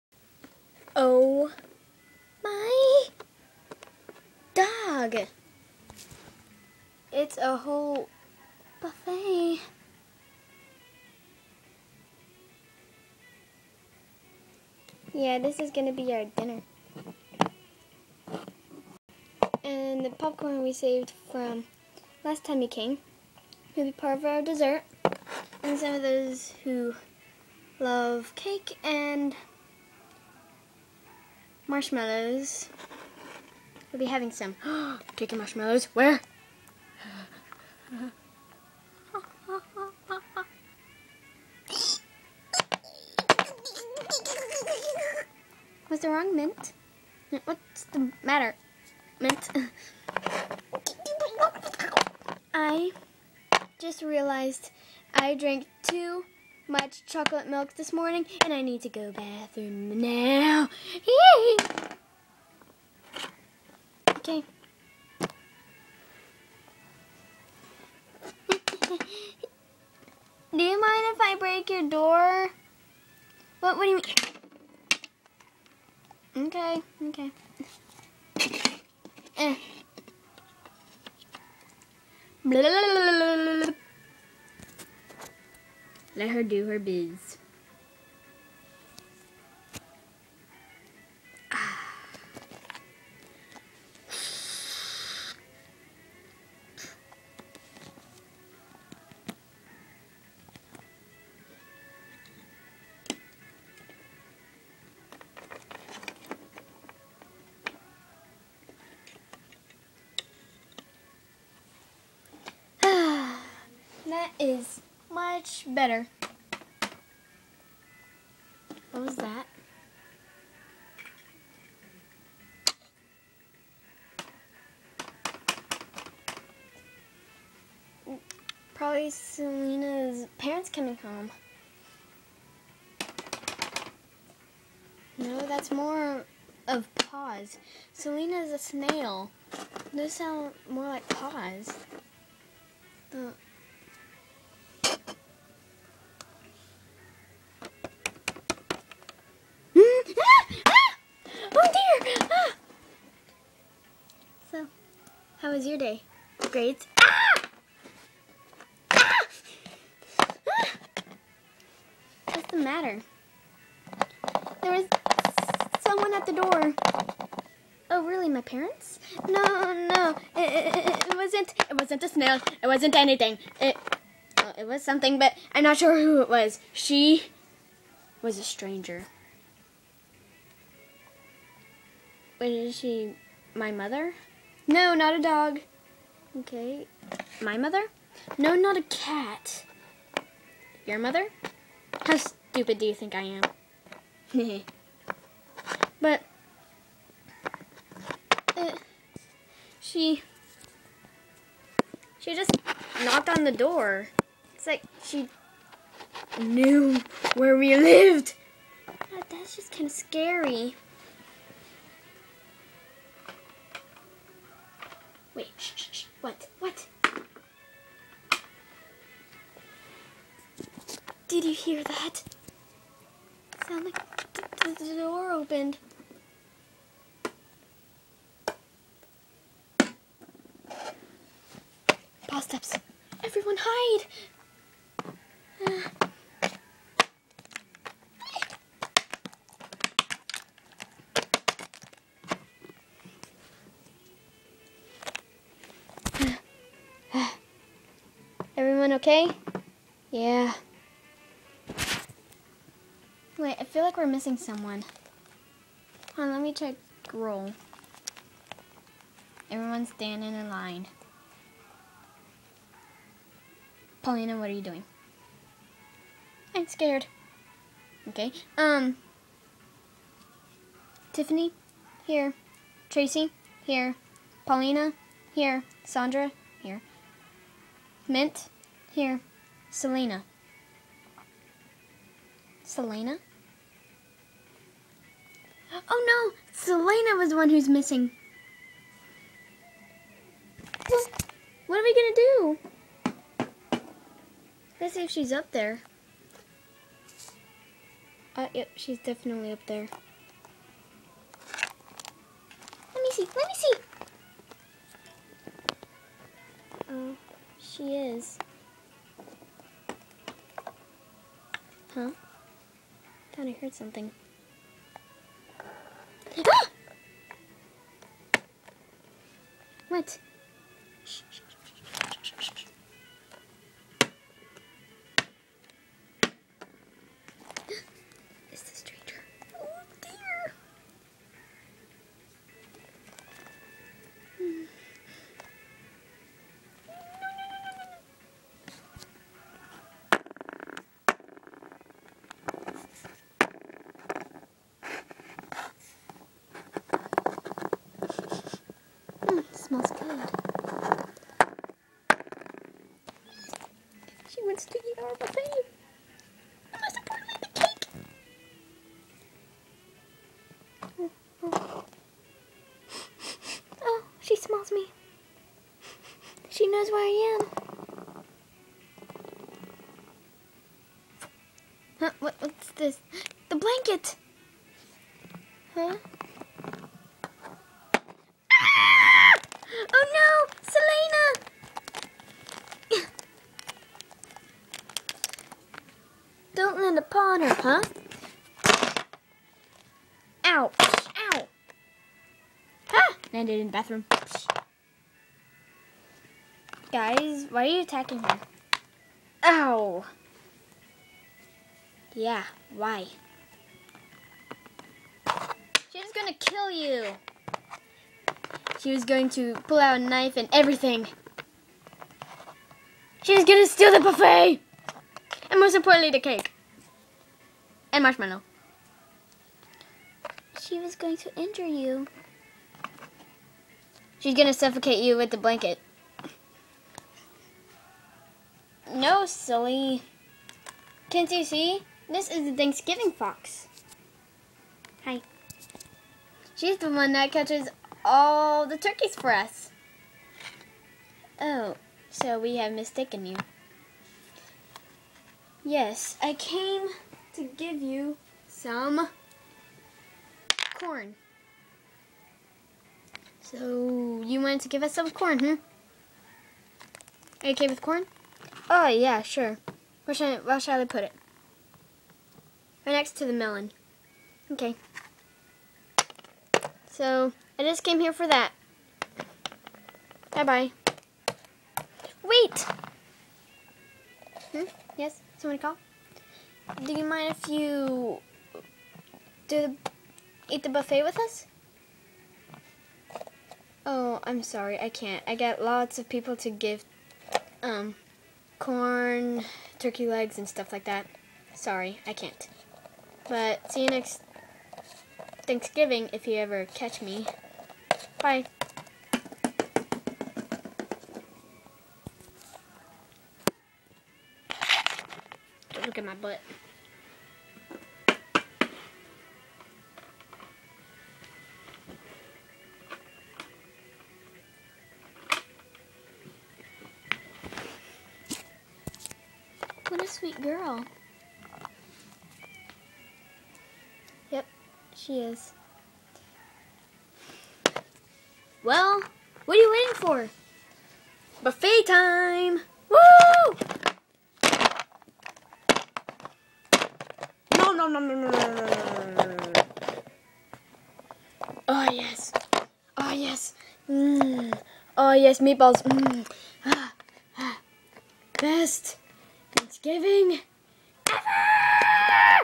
LPS Holiday Sleepovers Ep.2 ThanksGiving
Episode 2! LOL

Tiffany, Pollinah, Sandrah, Mint, and Tracy are all having a sleepover at Saleena's. Mint needs to go use it before she EXPLODES!!! LOL When she comes out, she here's a knock. Her and her friends haven't seen this stranger before. Who can it be?
Sorry for misspelling some names but its only episode 2.